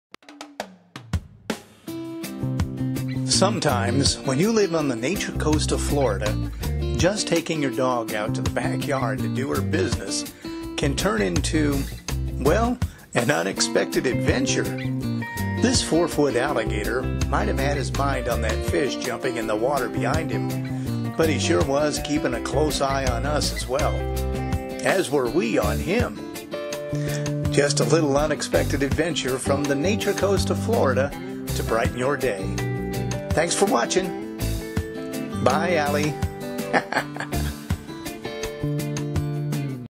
Unexpected Adventures - Call Me Al!
Unexpected Adventures with Vincent Autenrieb "Call me Al!"  July 2020  Encountered an Alligator in Back Yard.